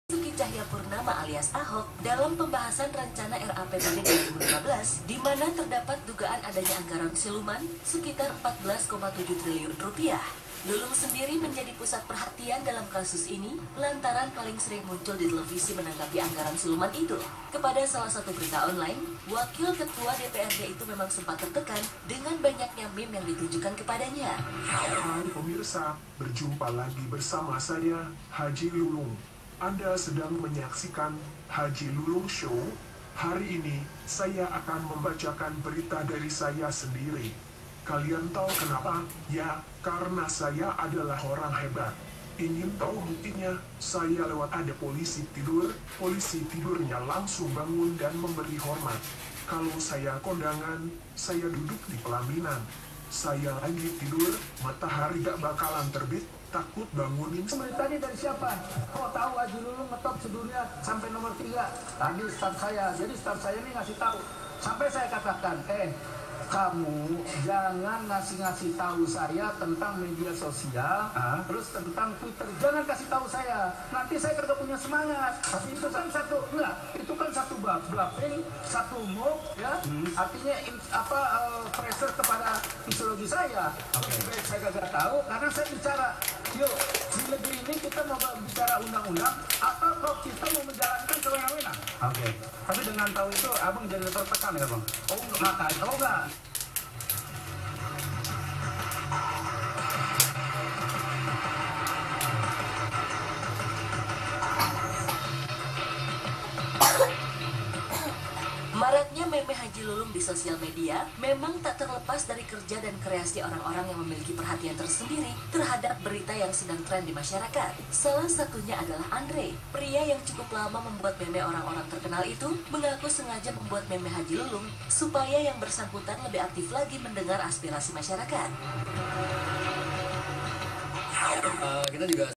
Meme Haji Lulung
Haji Lulung seorang wakil rakyat di Dprd Dki Jakarta menjadi terkenal setelah perseteruanya dengan Ahok. Maka muncullah di medsos meme haji lulung. Banyak kreatifitas meme haji lulung yang bisa anda lihat di video ini.